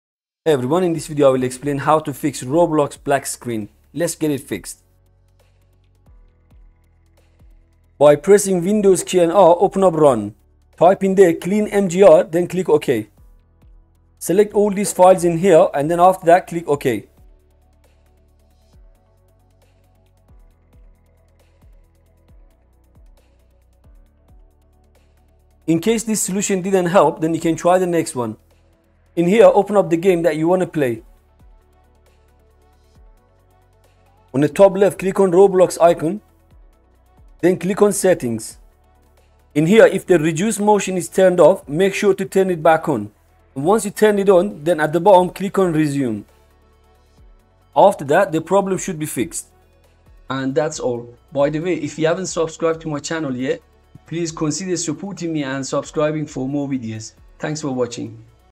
100% work "Roblox Black Screen Problem"
▶ How To Fix "Roblox Black Screen Problem" After Launch
"No More Roblox Black Screen After Launch - Easy Solution"

Learn in this video how to fix Roblox black screen after launch with our easy-to-follow solution!  Watch now and learn how to fix Roblox black screen issue after joining servers on iphone ipad and windows pc.